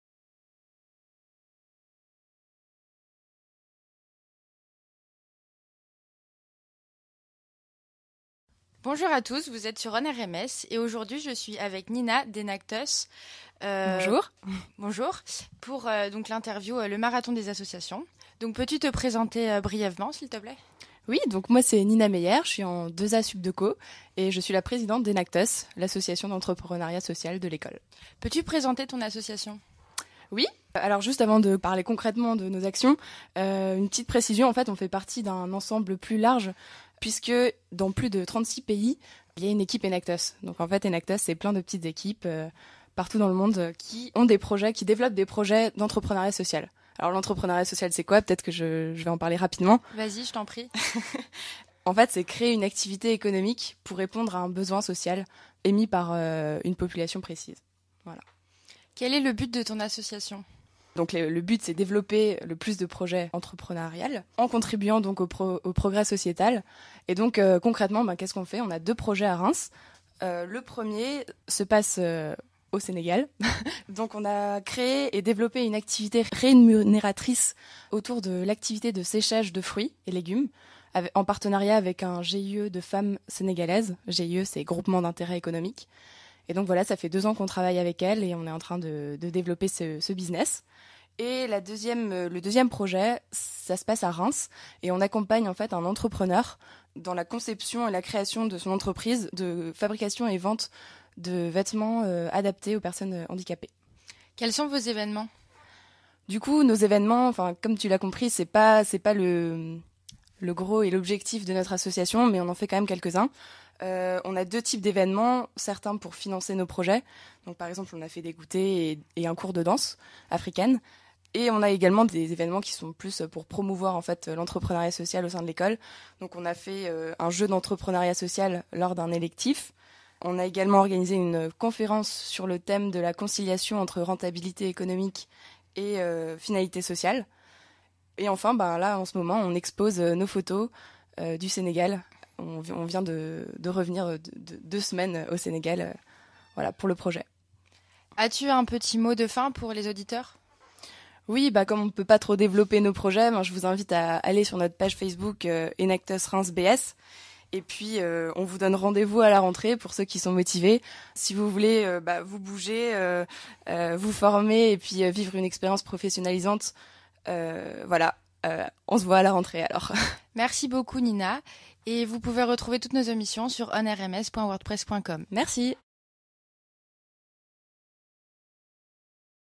Marathon des assos - Enactus
Présentation de l'association d'entreprenariat social de NEOMA Business School - Campus de Reims: Enactus.